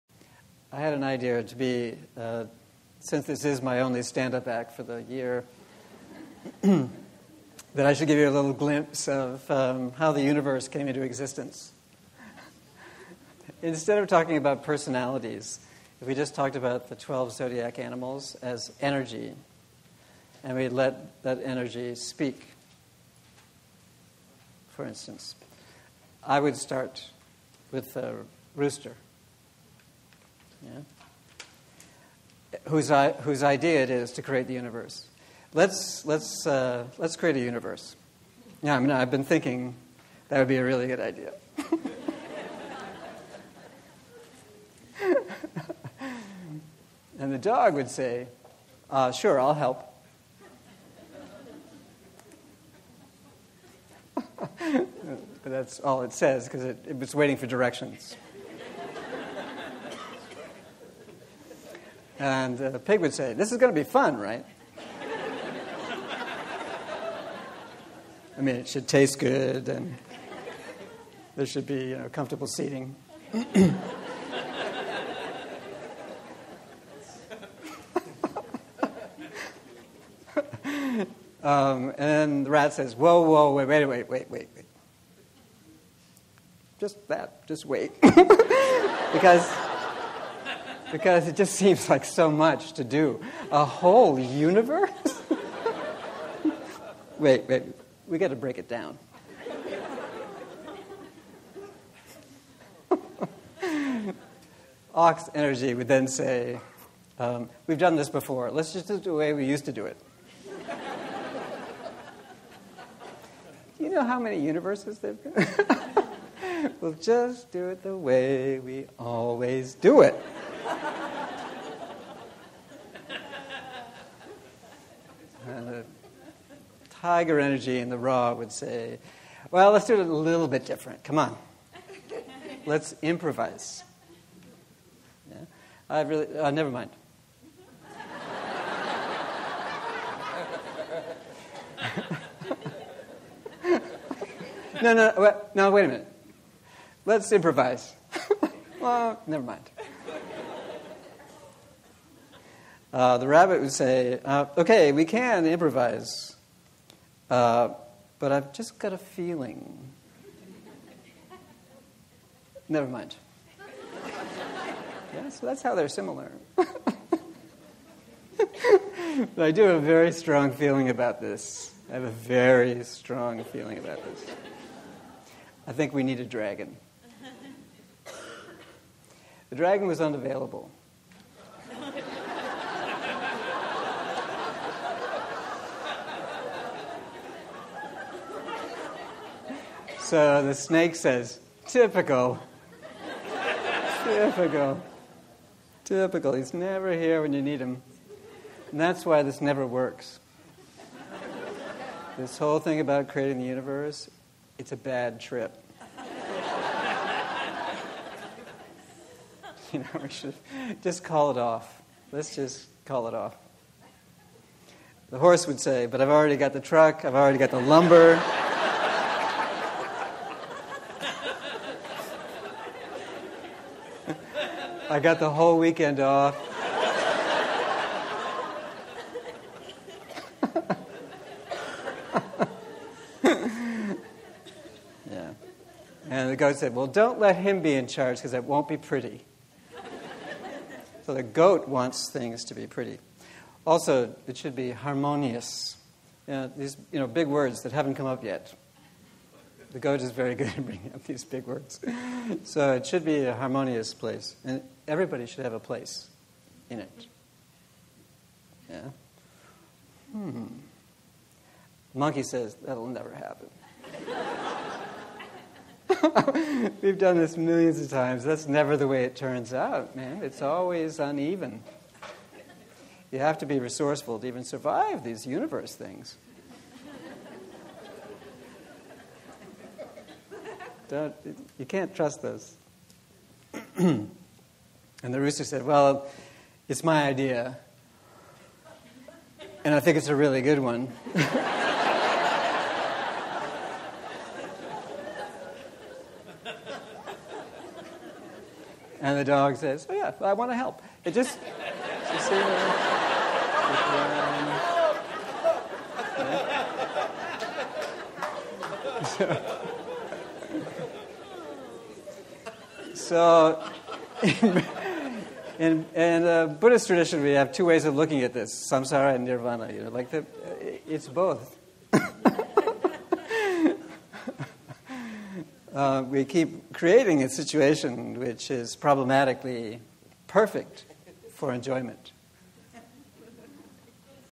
12 Chinese Zodiac Animals Generate a Universe
Every Chinese New Year (the first lunar month of Spring) Liu Ming holds a gathering in which he discusses the coming year's astrological influence according to the Chinese Song Dynasty Stems and Branches system of Astrology.  Proceeds from this popular talk are sent to support hand-picked projects in Central Asia.

For more on the Chinese and Central Asian Traditions visit DaYuanCircle